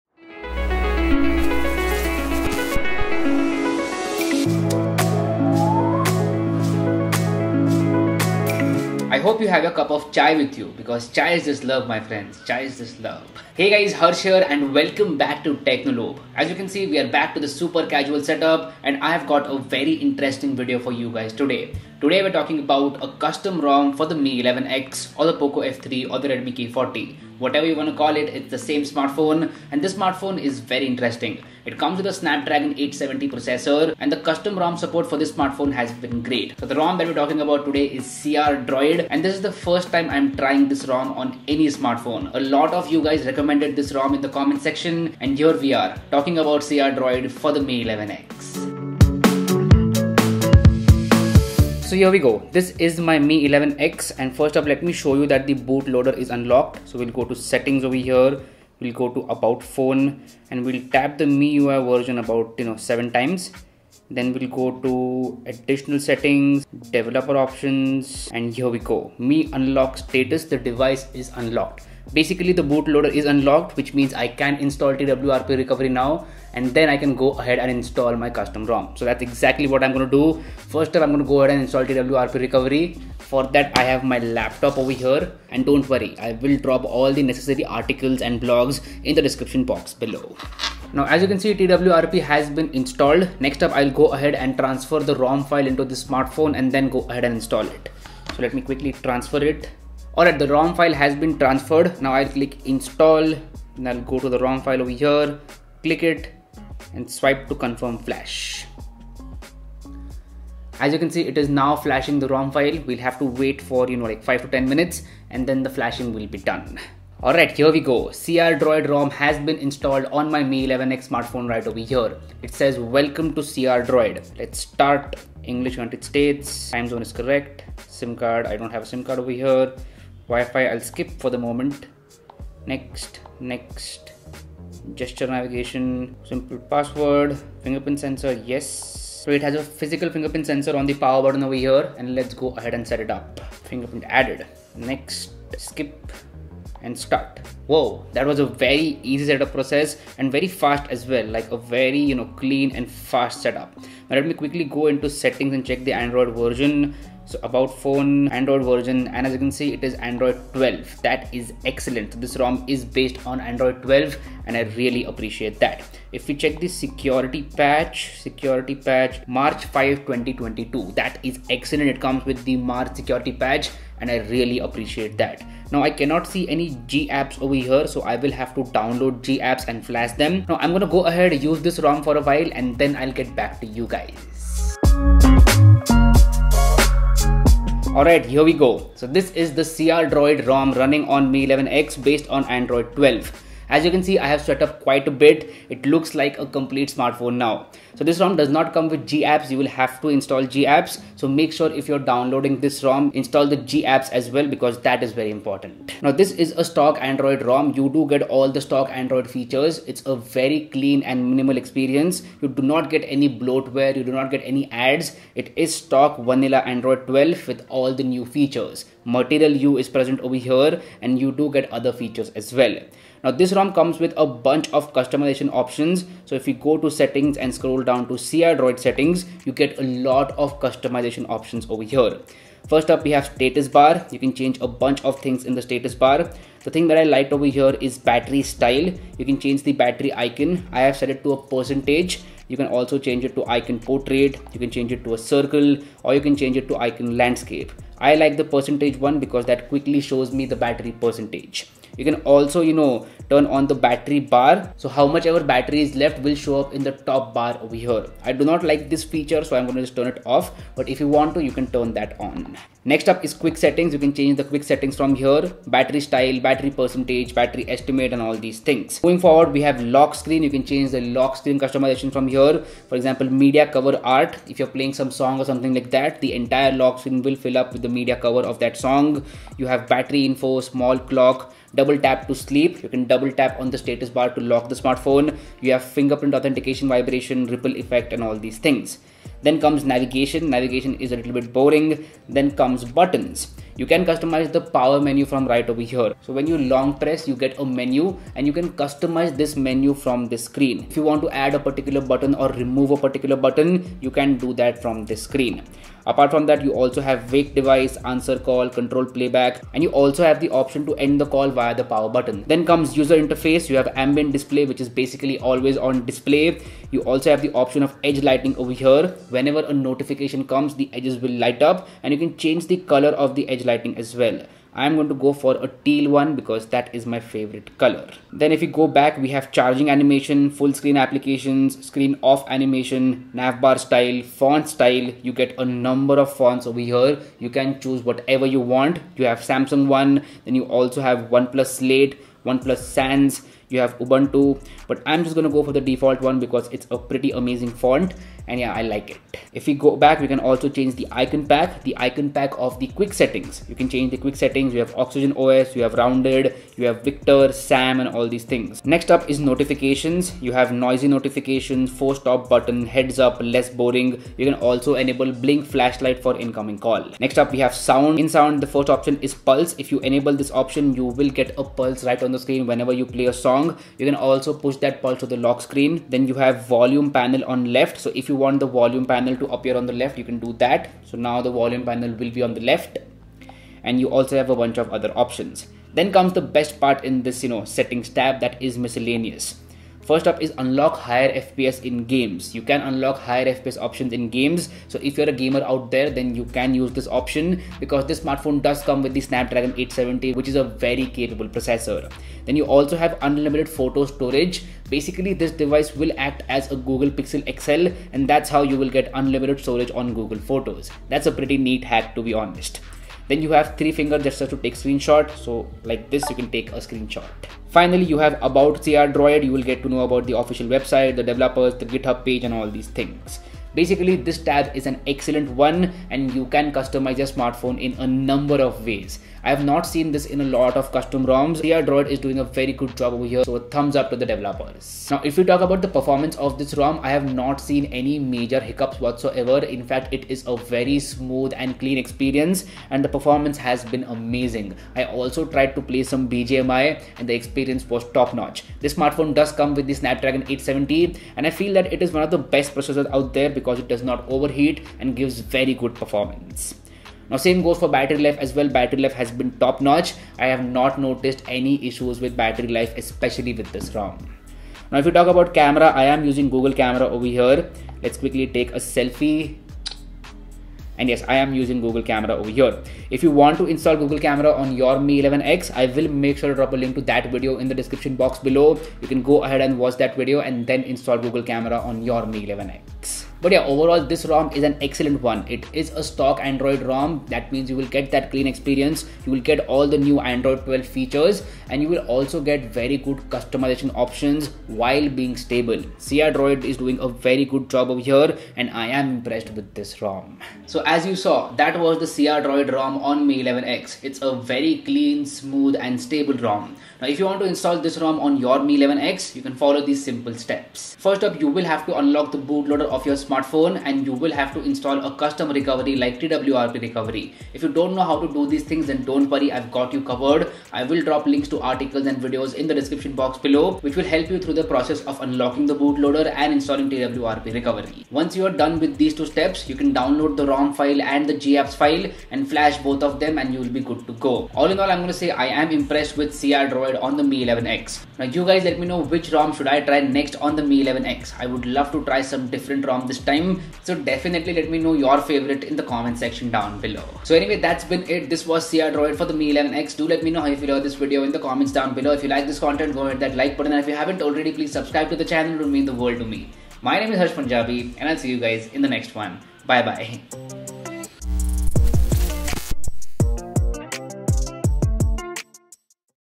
Mi 11X + CrDroid ROM = Insane!!
Mi 11X is an awesome smartphone and gets even better with the CrDroid Custom ROM. Today in this video let us have a look at the CrDroid Custom ROM for Mi 11X. I will show the installation process and we will also go over features of CrDroid based on Android 12.